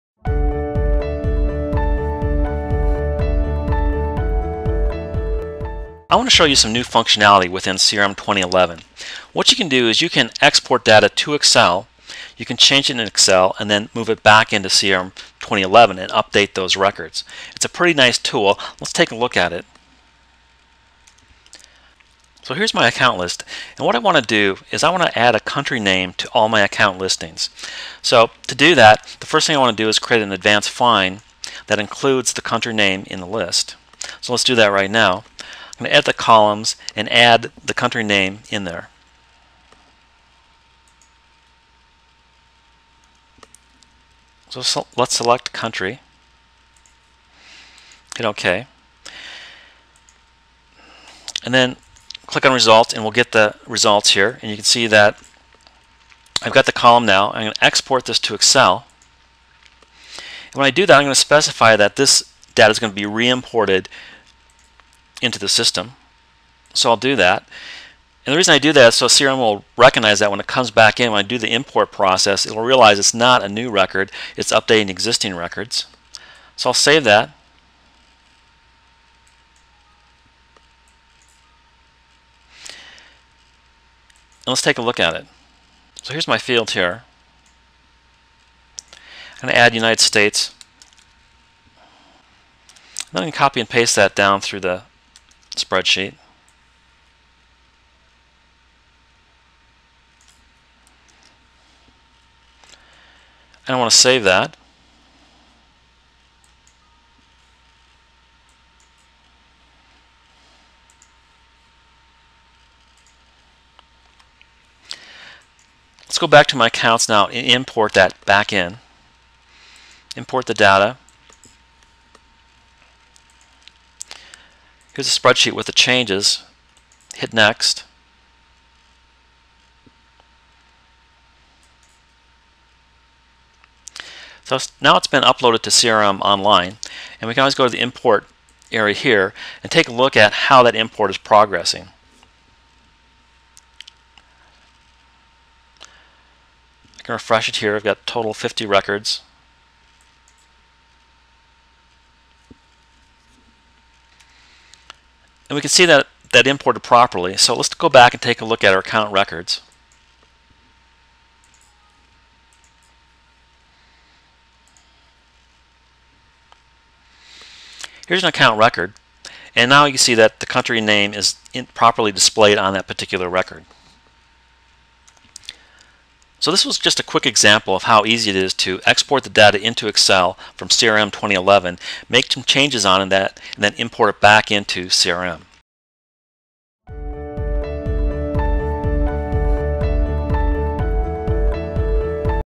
Excel Export and Import in Dynamics CRM
This is one of the neatest features released with CRM 2011. It allows you to export your CRM data into an Excel spreadsheet, make changes to fields, and then import them back into CRM. What's nice is that you can use the functionality in Excel to select, sort, and update records. For example you might use this feature to realign sales territories.